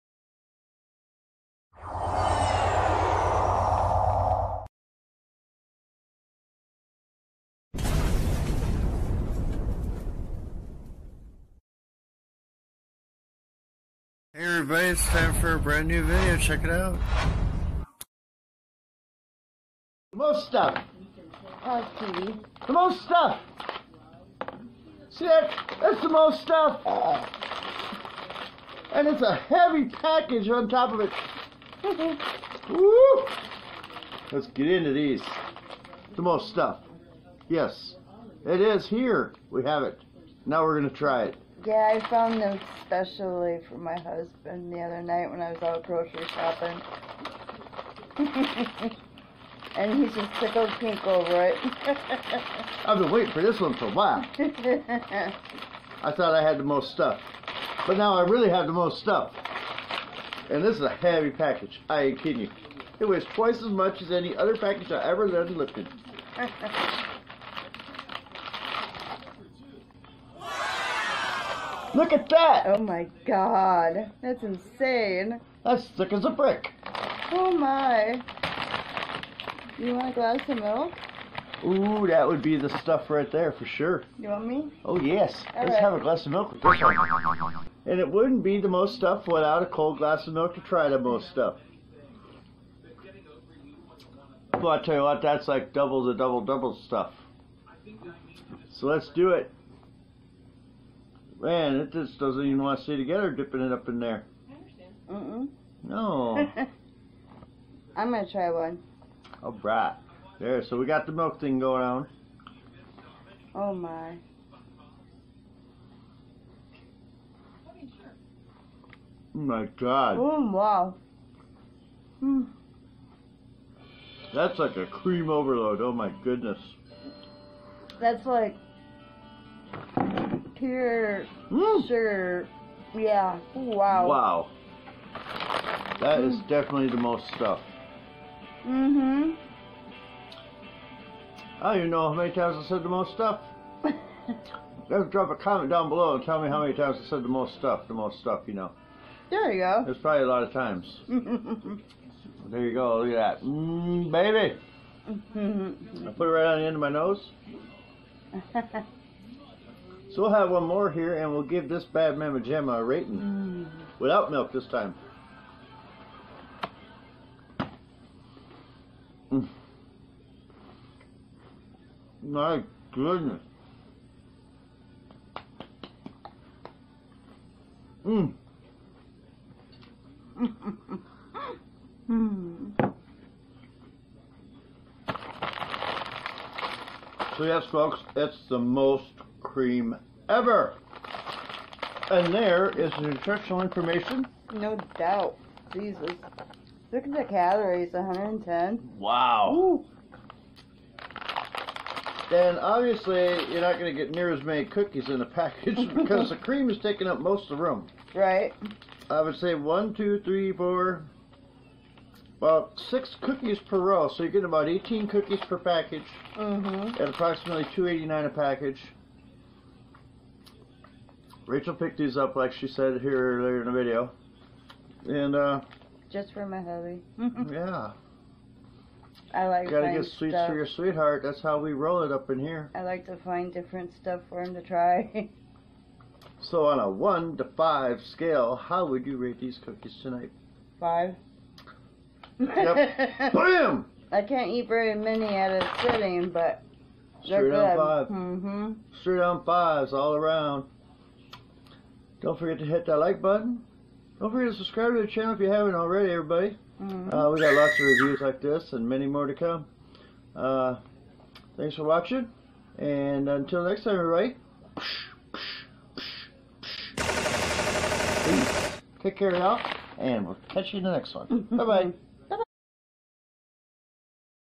Oreo The Most Stuf
New Limited Edition Oreo The Most Stuf
Most.Creme.Ever. These one of a kind cookies are extreme when it comes to the amount of creme filling!!!
Thanks4Watchin